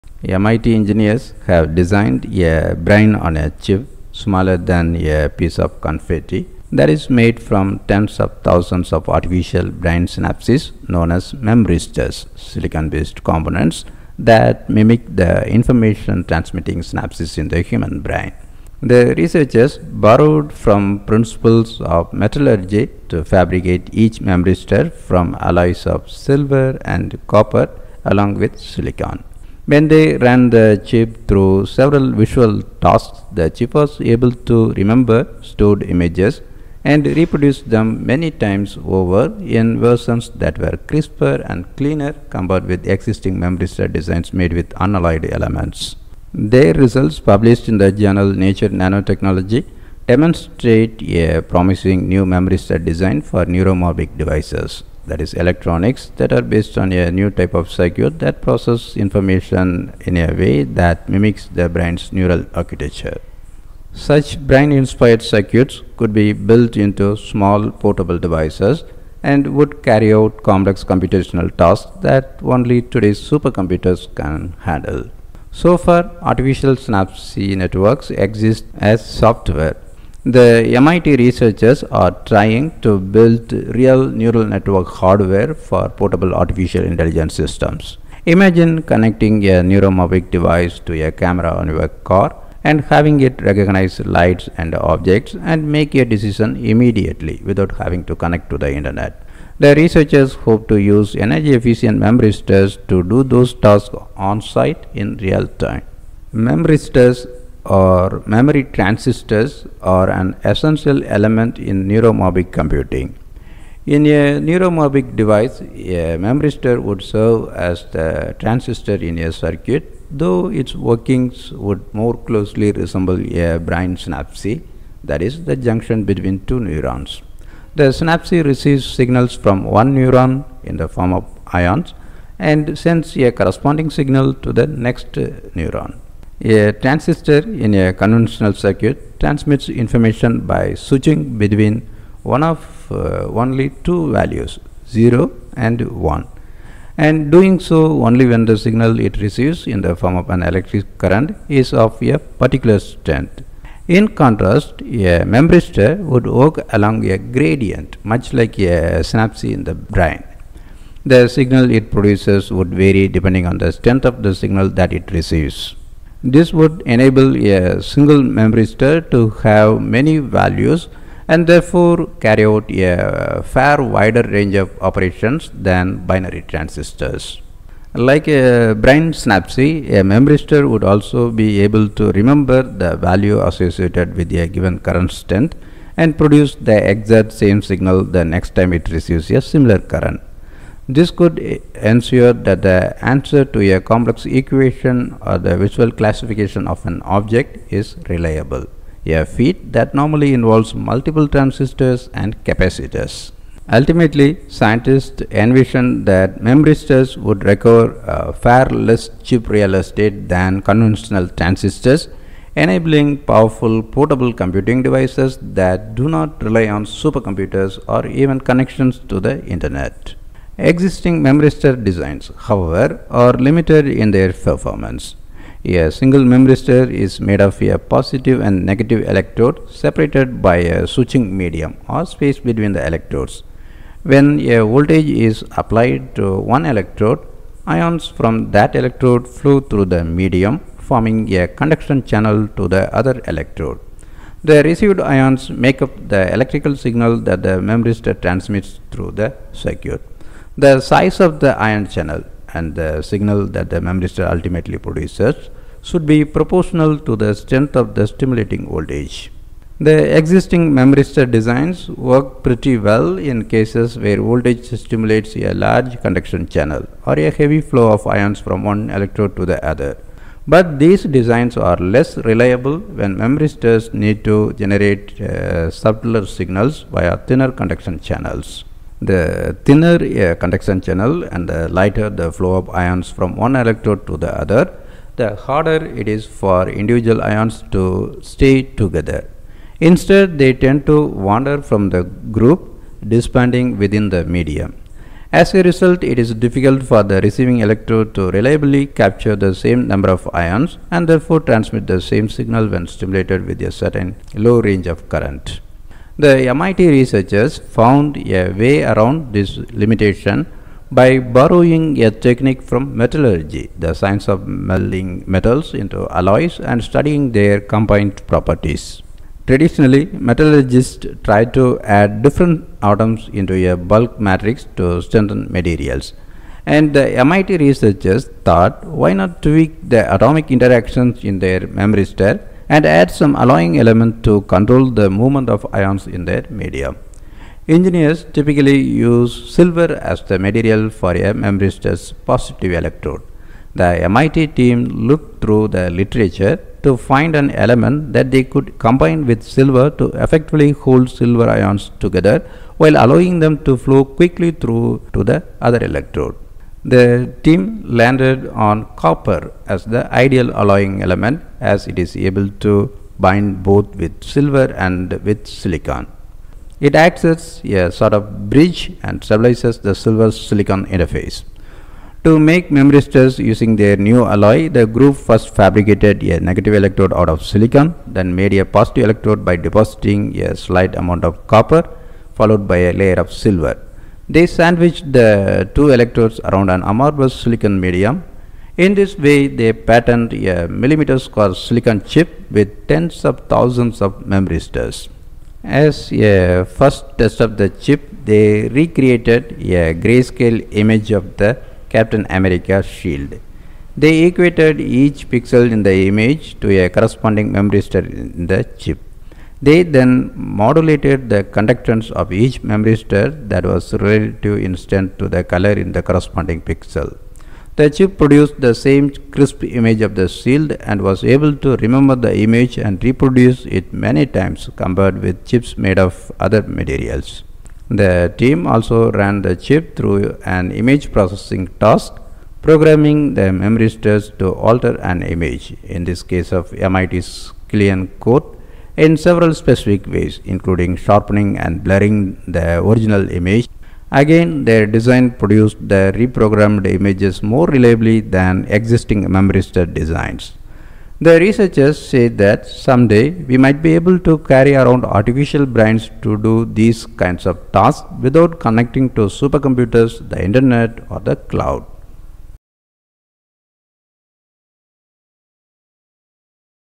Brain-on-a-chip | Engineers put tens of thousands of artificial brain synapses on a single chip
MIT engineers have designed a “brain-on-a-chip,” smaller than a piece of confetti, that is made from tens of thousands of artificial brain synapses known as memristors — silicon-based components that mimic the information-transmitting synapses in the human brain.

Credit: Peng Lin
Credit: MIT